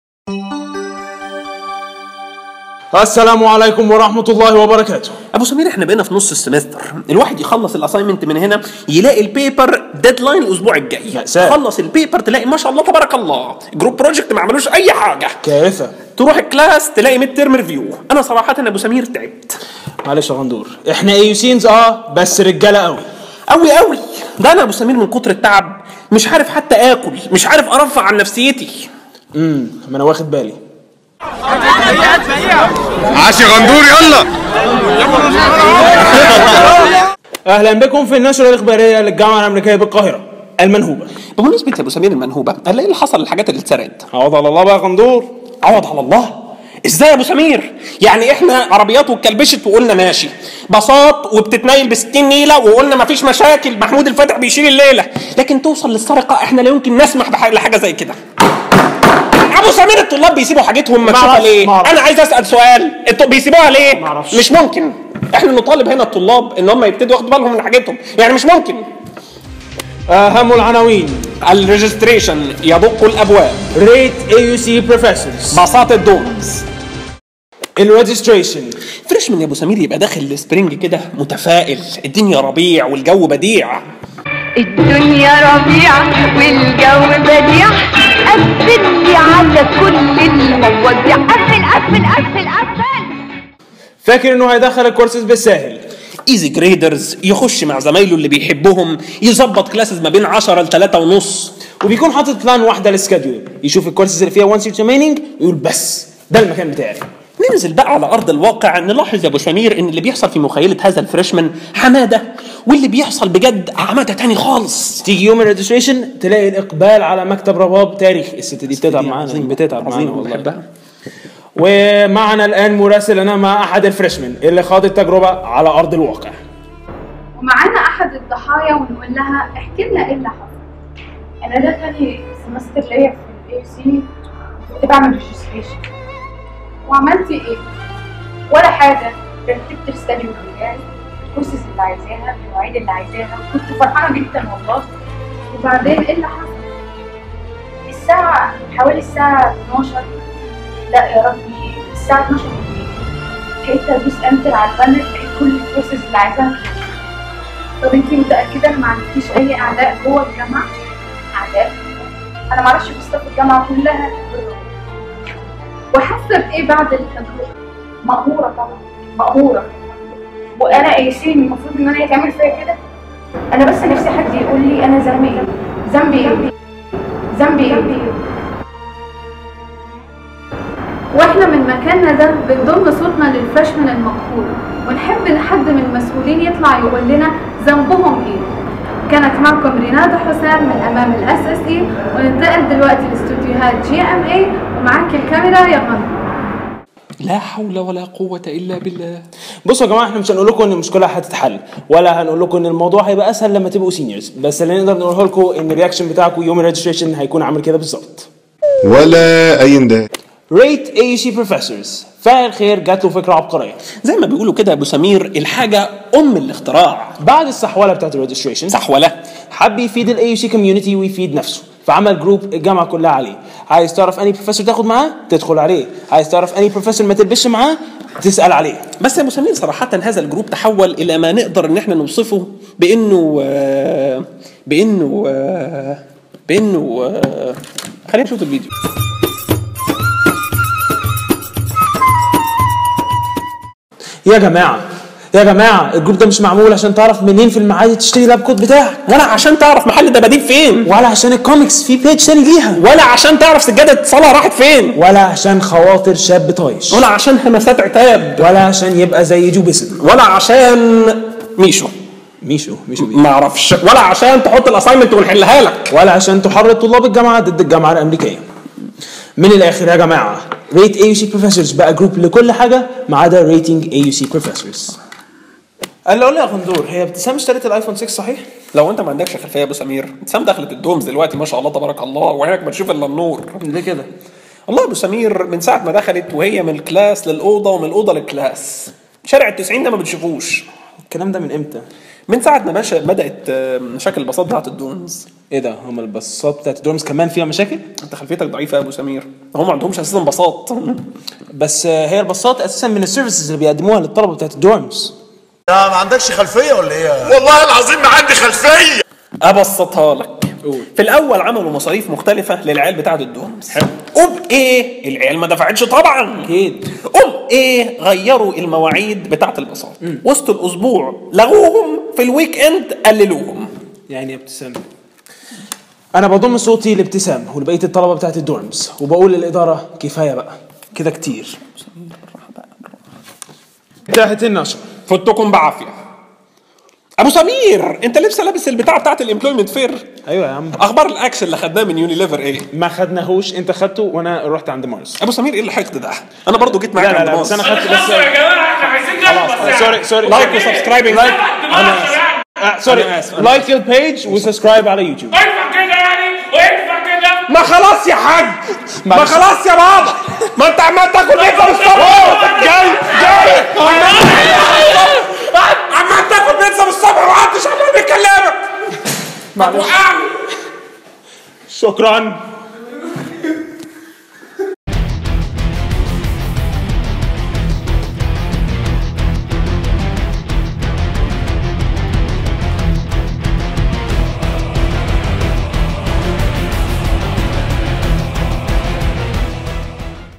Good Morning AUC - S01E05
Good Morning AUC team is still hanging tight despite finals!
-Registration, really? You suffered, we delivered!
-Rate AUC Professors - Where everything is possible
-Buses seem to stalk us! We've got our dorms to stand up for!